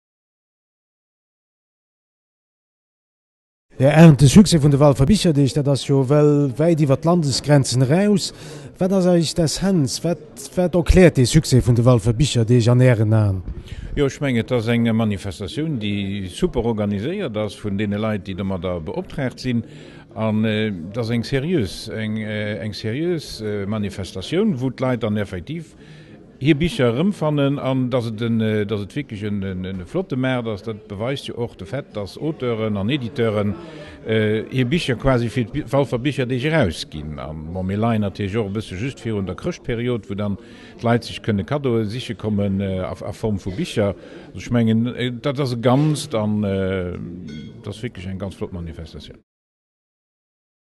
Walfer Bicherdeeg 19 et 20 novembre 2011: Interview du Bourgmestre de Walferdange, Guy Arendt
[HD2011-09]